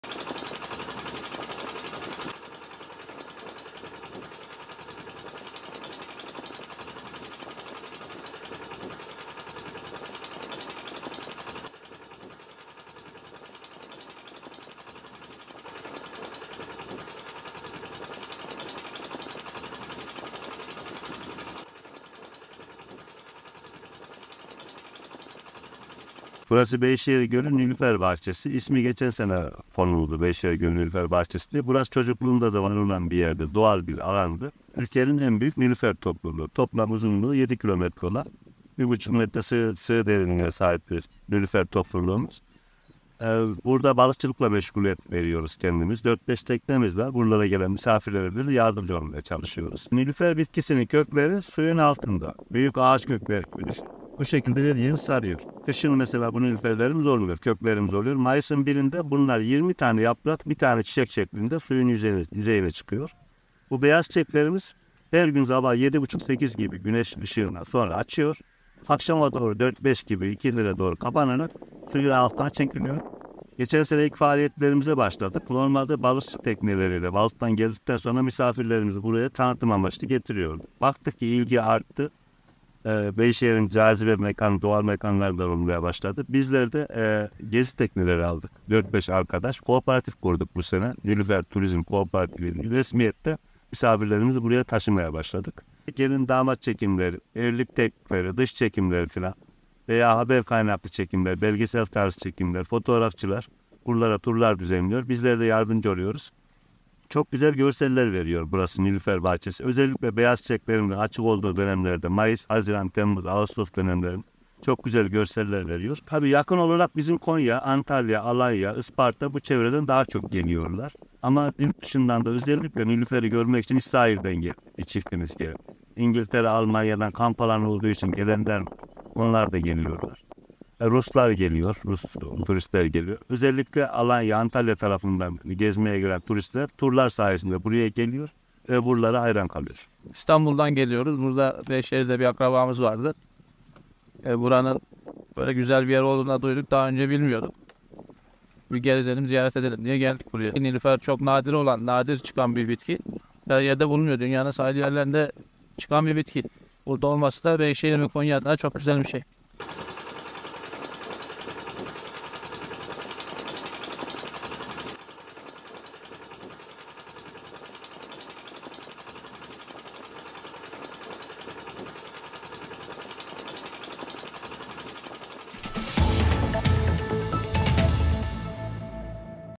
Beyşehir Gölü'nde balıkçı tekneleriyle gezilen nilüferler için tekne turları düzenleniyor
- Nilüfer Turizm Kooperatifi yöneticisi İbrahim Erdoğan:
- "Balıkçı tekneleriyle balıktan geldikten sonra misafirlerimizi buraya tanıtım amaçlı getiriyorduk. Baktık ki ilgi arttı"
- "Burası, Beyşehir'in cazibe ve doğal mekanlarından olmaya başladı. Gezi tekneleri aldık. Buradaki arkadaşlarla Nilüfer Turizm Kooperatifini kurduk"